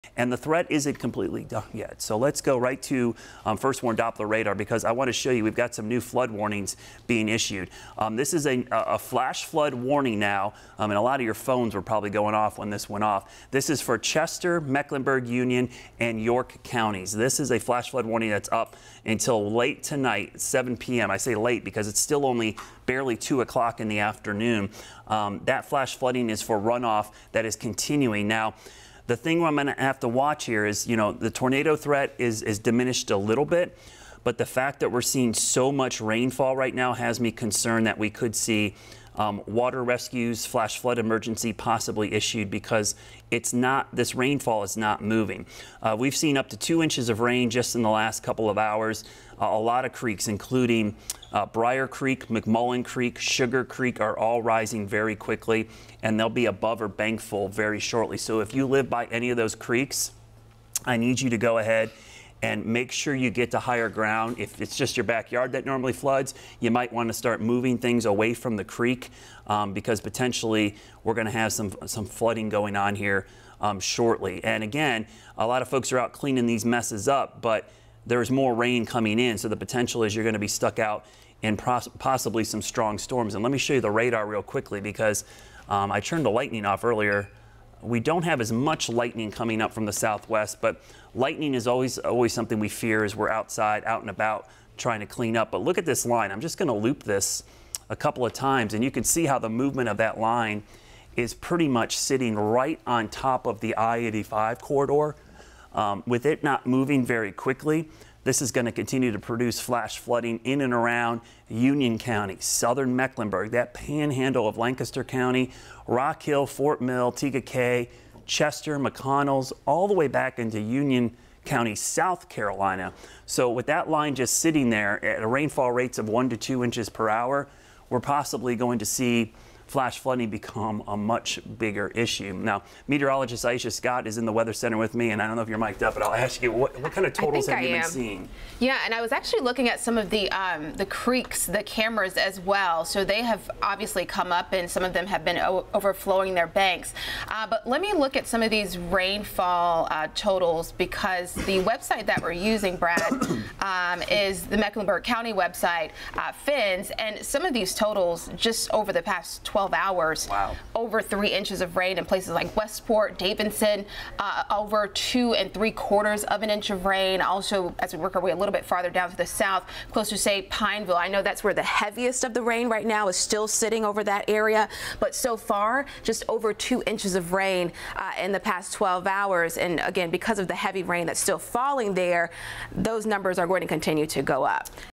Flash Flood Warning issued for Charlotte, North Carolina
The entire Charlotte area is under a Flash Flood Warning after a line of severe storms moved through the region Thursday morning. The warning is in effect until 7 p.m. Thursday with flash flooding that could run off from earlier rain. The tornado threat has diminished for the area, but heavy rain could lead to water rescues and downed trees. The Charlotte area has seen up to 2 inches of rain in only about two hours.